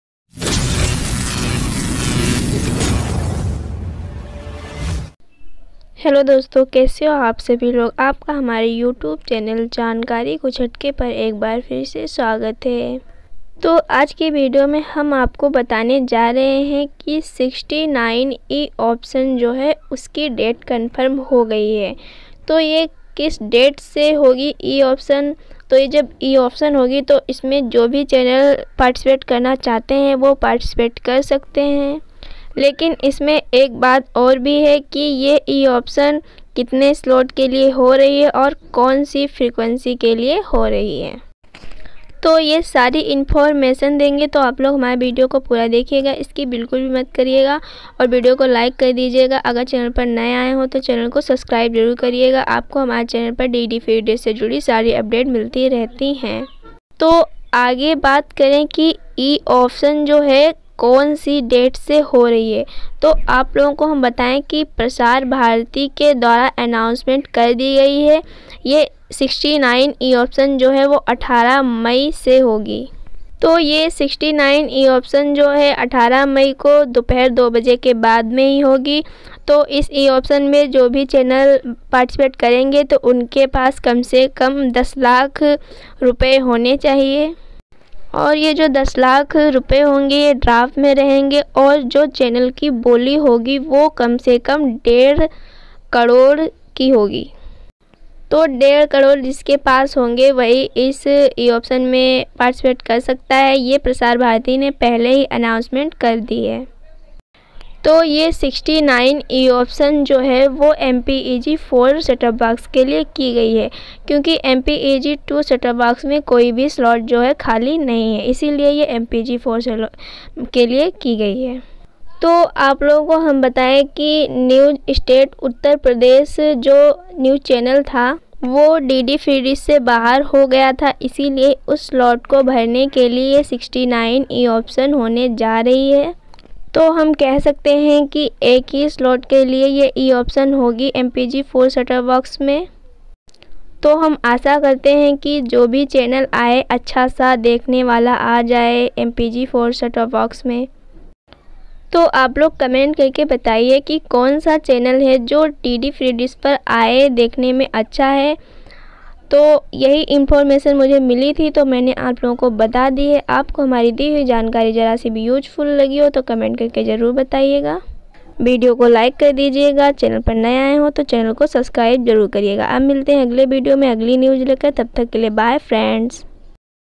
69 E Auction Date Confirm MPEG 4 Set up box/New Update Today /18 May Annausment Date ❤️❤️
हमने इस वीडियो में यह बताया कि डीडी फ्री डिश पर 69 e auction date confirm 😄😄🙏🙏